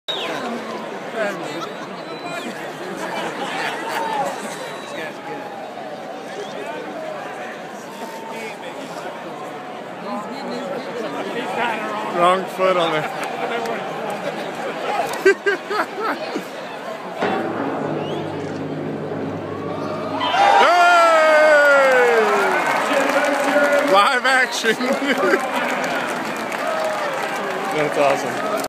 Turtleman Throws out first pitch at Reds game
Live Action!

The Turtleman from The Call of the Wildman Tv show throws out 1st pitch for the Reds v. Angels game!